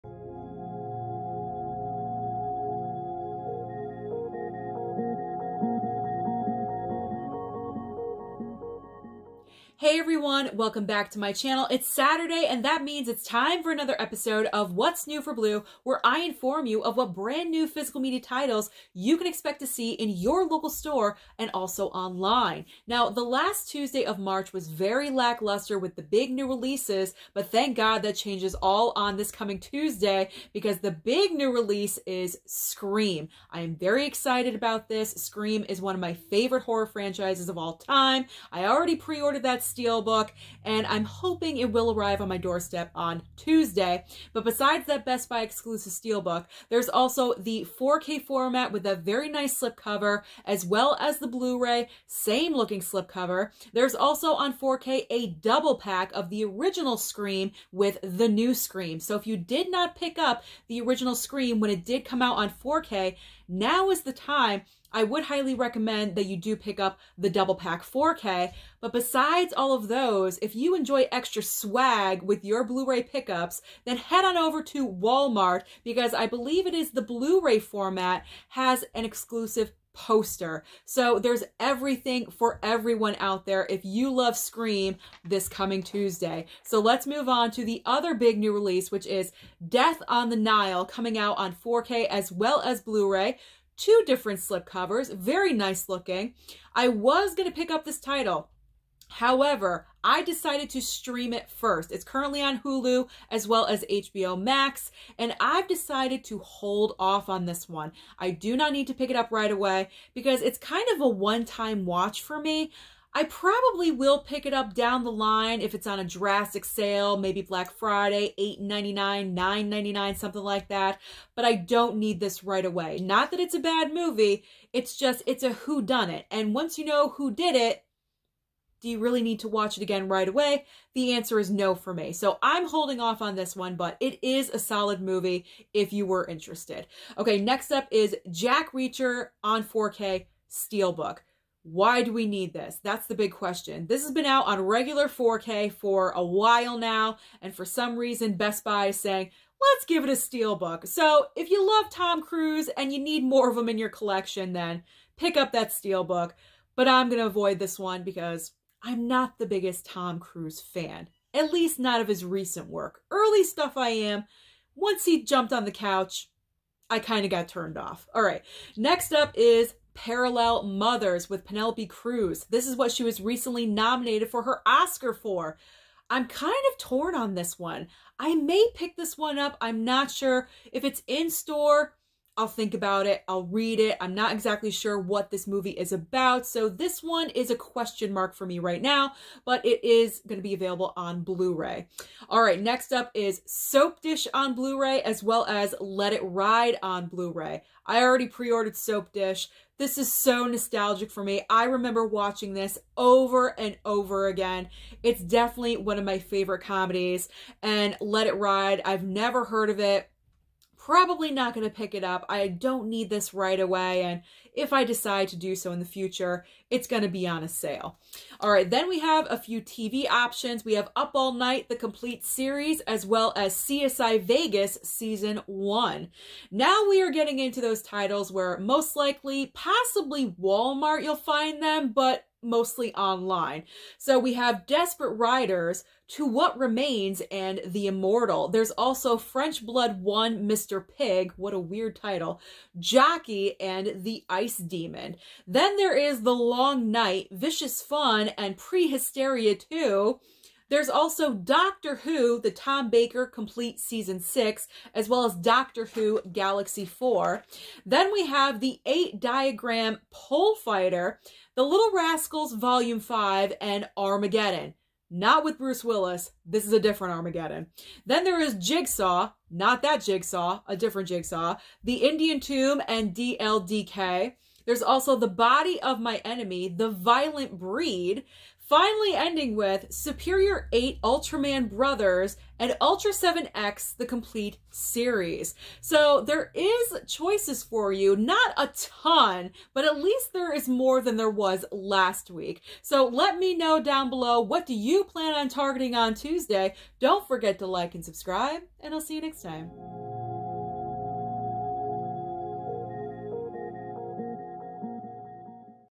What's New For Blu? - Ghostface, Death On The Nile and an Oscar Nominee!
April starts off with a bang that will make you go running to your local stores! The big new releases include Scream, Death On The Nile, Jack Reacher Steelbook and MORE!!! Grab your wallets and prepare to spend!

Every week I'm going to update you on what movies are being released Tuesday, on what format and where you can purchase them!

New Releases:
Scream
Death On The Nile
Jack Reacher
Parallel Mothers
Soapdish
CSI:Vegas
Desperate Riders
To What Remains
The Immortal
French Blood 1: Mr. Pig
Jockey
The Ice Demon
The Long Night
Vicious Fun
Prehysteria! 2
Doctor Who: Tom Baker
Doctor Who: Galaxy 4
The 8 Diagram Pole Fighter
The Little Rascals Vol 5
Armageddon
Jigsaw
The Indian Tomb
2LDK
The Body Of My Enemy
The Violent Breed
Superior 8 Ultraman Brothers
Ultraseven X: The Complete Series

What are YOU picking up this week?

Letterboxd - susansreviews

Fan Mail can be sent to:
Susan Oakes